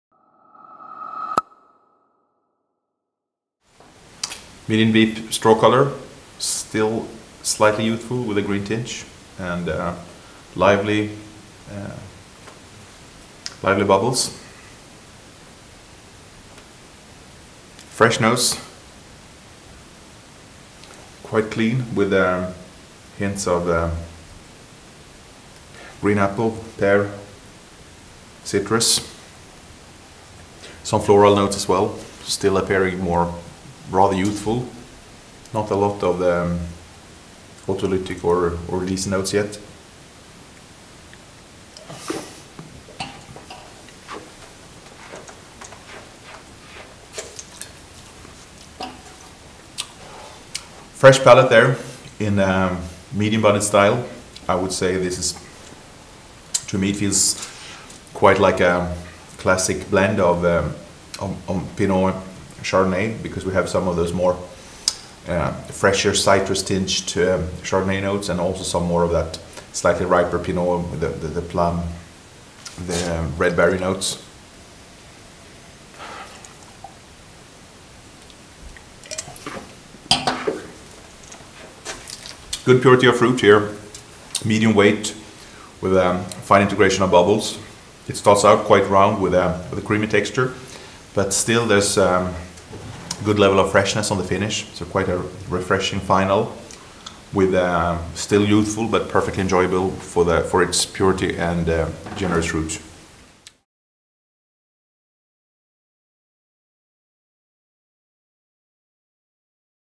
Champagne Charles Mignon - Brut Premium Reserve NV - Premier Cru
Medium deep straw colour, still slightly youthful with a green tinge and lively bubbles. Fresh nose, quite clean; with hints of green apple, pear, citrus, some floral notes as well. Still appearing rather youthful, not a lot of autoystic or leesy notes yet. Fresh palate there, in a medium body style. I would say that to me it feels quite like a classic blend of Pinot and Chardonnay, because we have some more freshness citrus tinge Chardonnay notes and also some slightly riper Pinot, the plum, the red berry notes. Good purity of fruit here, medium weight. With a fine integration of bubbles. It starts out quite round with a creamy texture, but still there is a good level of freshness on the finish. Good level of freshness on the finish, quite a refreshing final. Still youthful, but perfectly enjoyable for its purity and generous fruit.